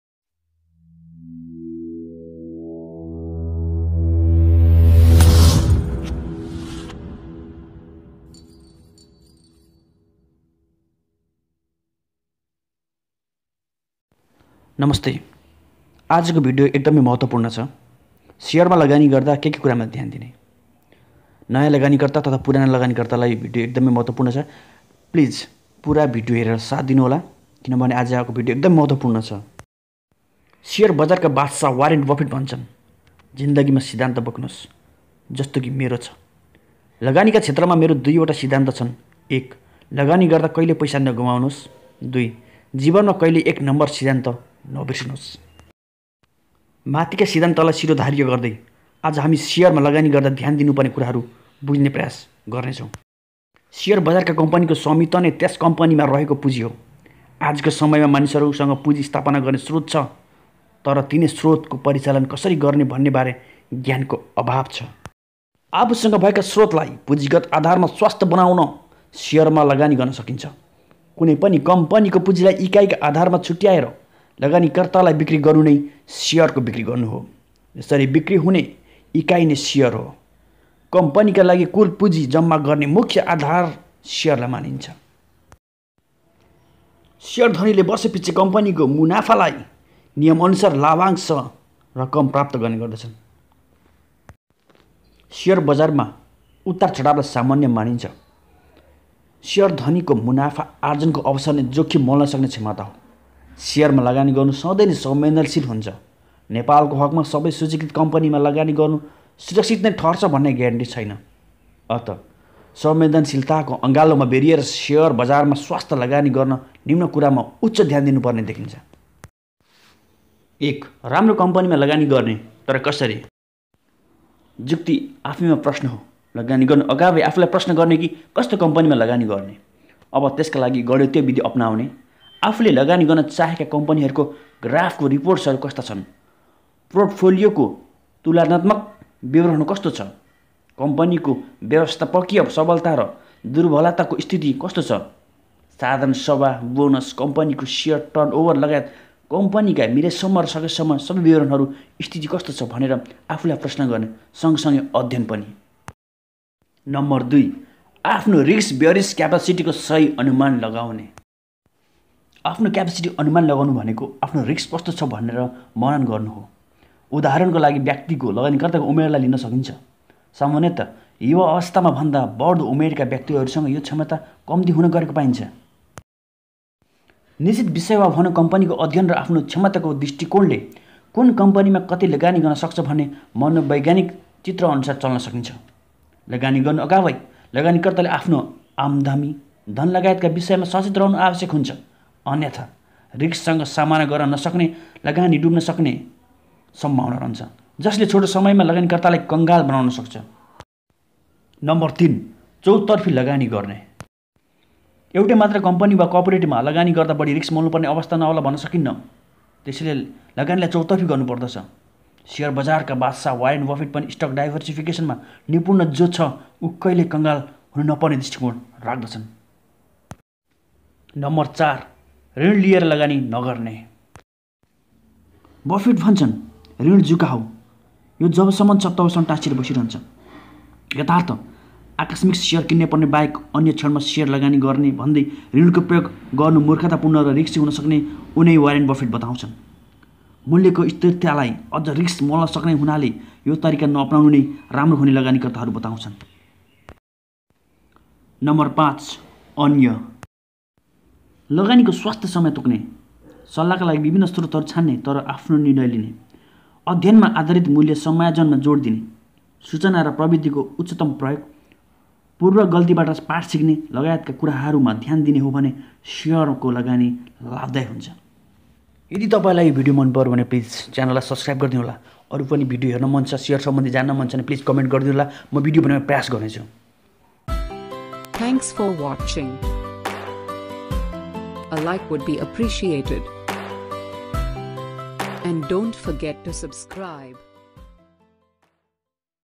nepal share market// Share market// Nepali share bazar// know to invest in share market in nepal
Namasta
जुक्ति आफैँमा प्रश्न हो | लगानी गर्नु अगावै आफैँलाई प्रश्न गर्ने कि कस्तो कम्पनीमा लगानी गर्ने ?

अब त्यसका लागि गणितीय विधि अपनाउने | आफूले लगानी गर्न चाहेका कम्पनीहरूको ग्राफको रिपोर्टहरू कस्ता छन् ? पोर्टफोलियोको तुलनात्मक विवरण कस्तो छ ? कम्पनीको व्यवस्थापकीय सबलता र दुर्वलताको स्थिति कस्तो छ ? साधारण सभा, बोनस, कम्पनीको शेयर टर्नओभर लगायत कम्पनीका मिलेसम्म र सकेंसम्म सबै विवरणहरूको स्थिति कस्तो छ ? भनेर आफैंलाई प्रश्न गर्ने | सँगसँगै अध्ययन पनि |

निश्चित विषय वा भनौं कम्पनीको अध्ययन र आफ्नो क्षमताको दृष्टिकोणले कुन कम्पनीमा कति लगानी गर्नसक्छ भन्ने मनोवैज्ञानिक चित्रनुसार चल्न सकिन्छ | लगानी गर्नु अगावै लगानीकर्ताले आफ्नो आम्दानी, धनलगायतका विषयमा सचेत रहनु आवश्यक हुन्छ | अन्यथा रिस्कसँग सामना गर्न नसके लगानी डुब्न सक्ने सम्भावना रहन्छ, जसले छोटो समयमै लगानीकर्तालाई कंगाल बनाउन सक्छ |
Jay nepal jay share
Comment garnu hola aru pani video herna maan vaya i will try my best to bring moreeeeee videos